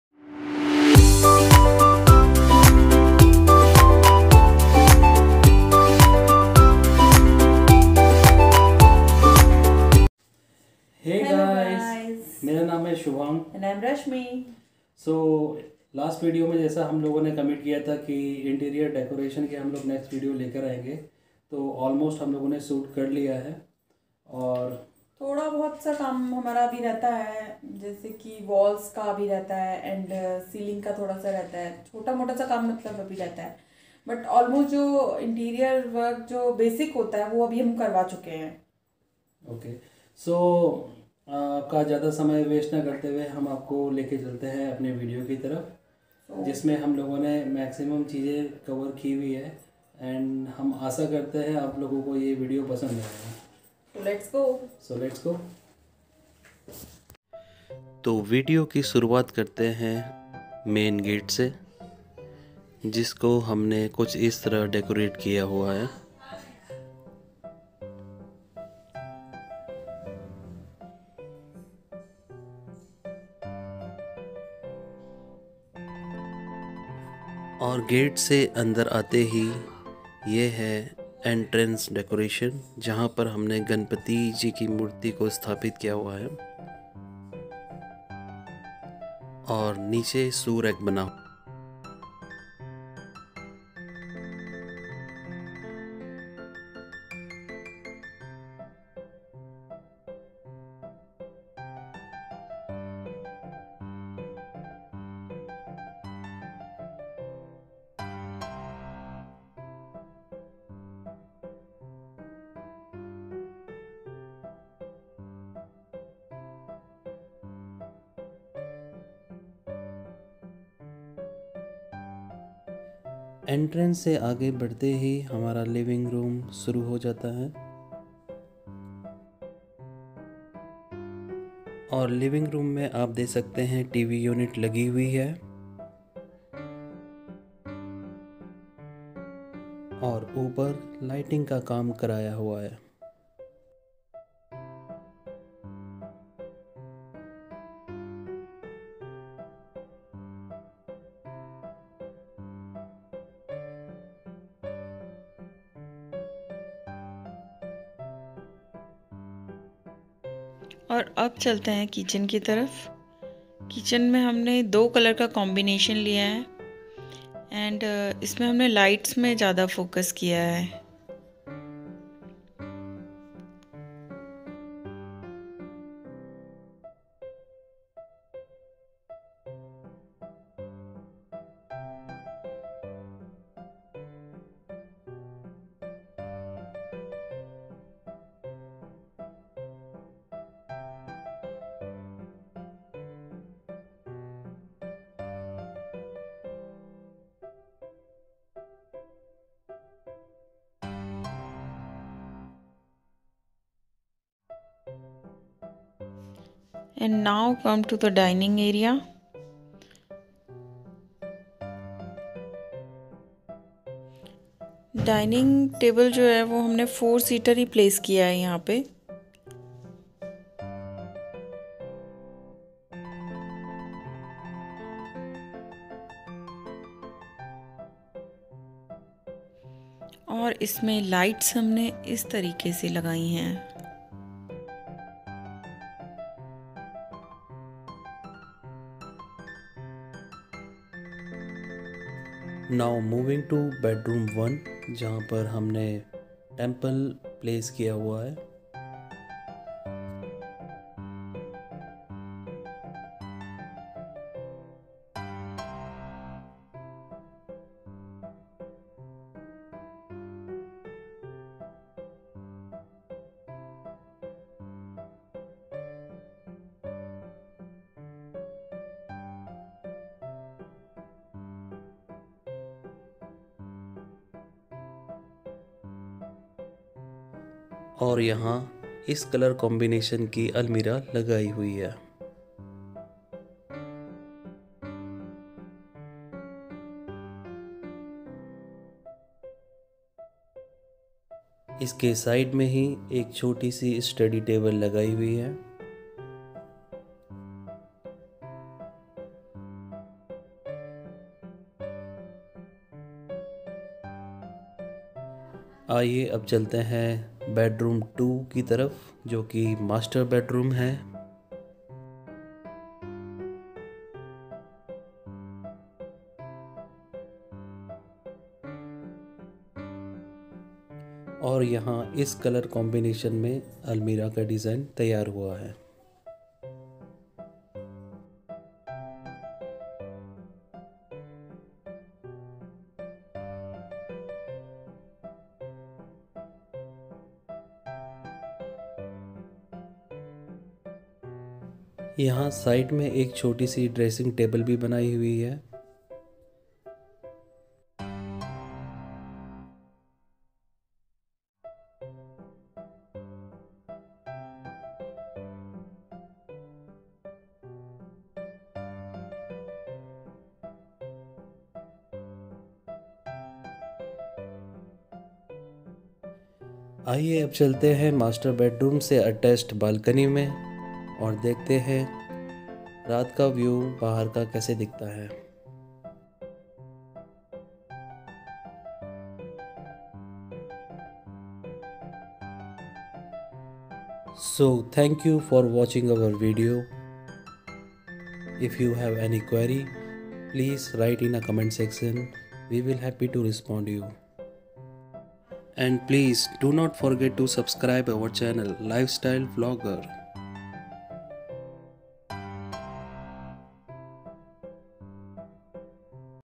Home Tour Video🏡
Hey everyone,
As committed in our previous video, we are now happy to share the home tour of our flat after finishing of all wooden work.
We hope you will like the video and if you have any suggestion or query, please write down in the comment section.
We will happy to respond you.

Subhang and Rashmi🙏